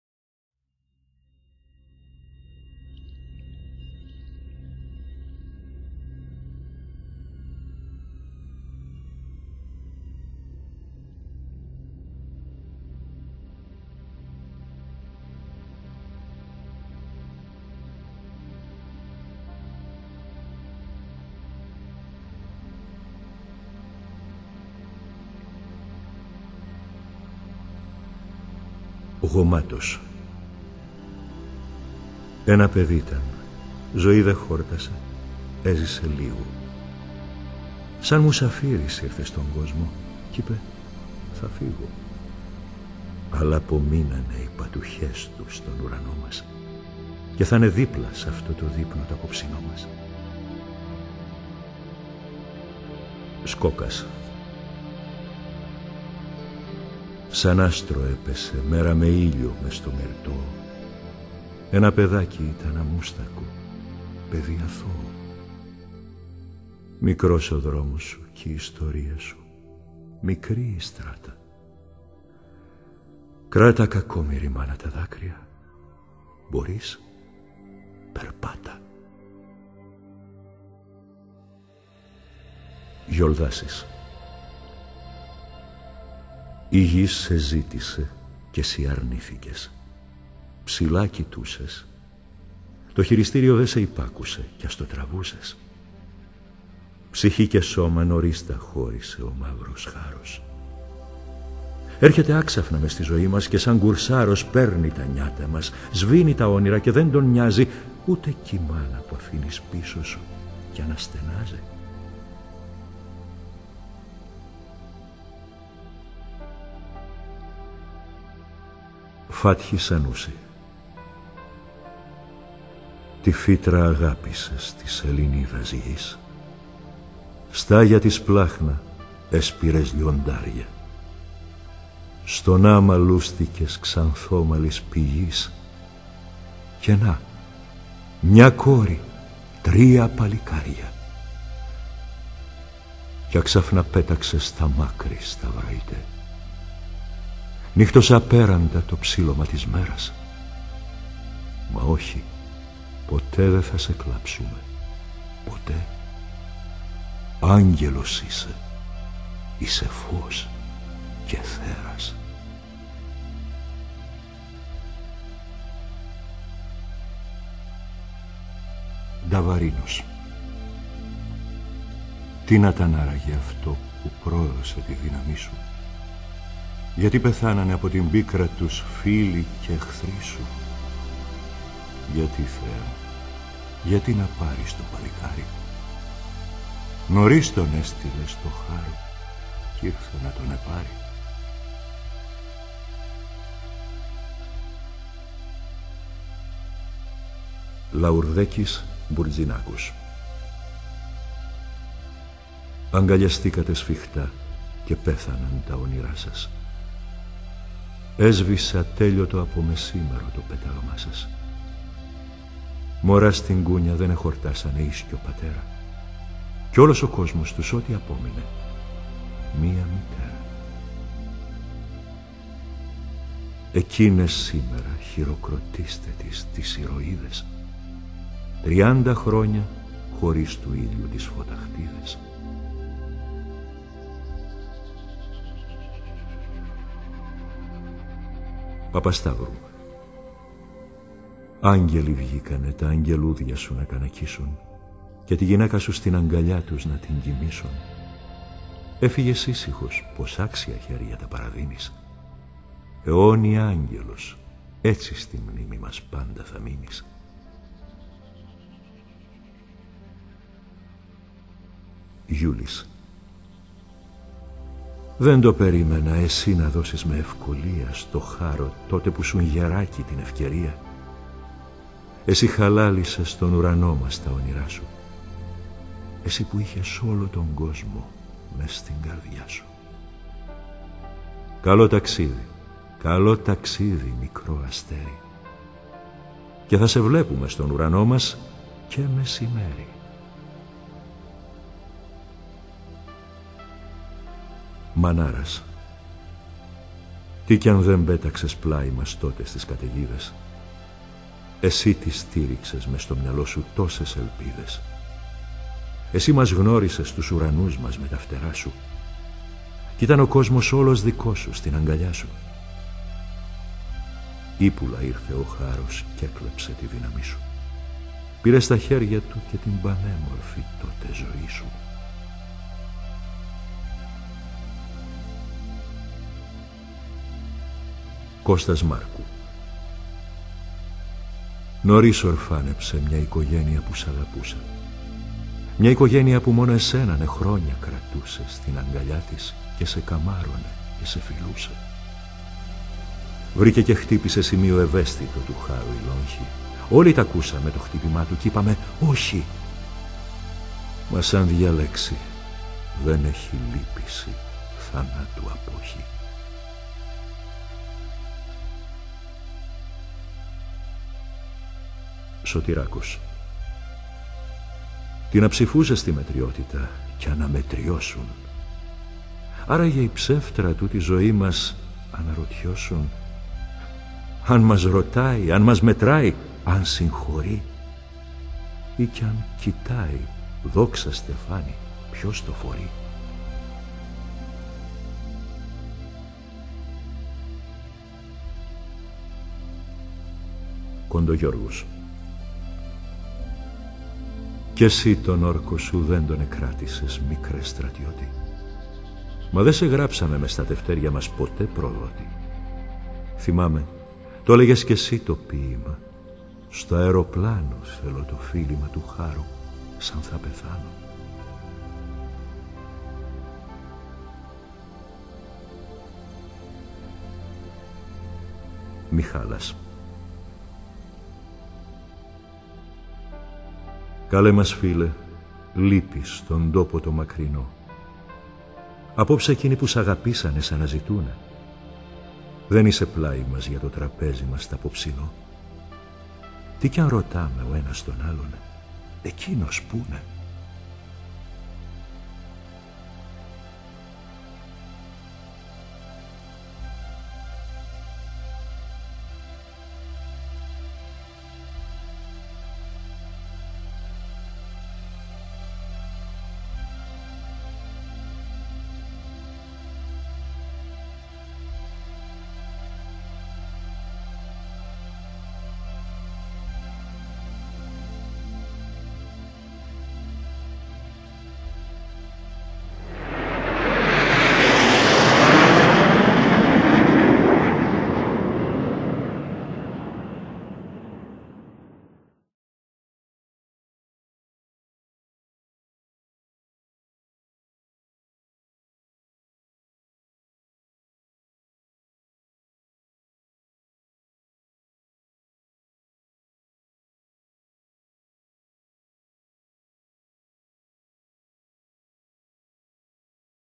Ikaroi tribute
This is a video tributing the loss of some greek pilots.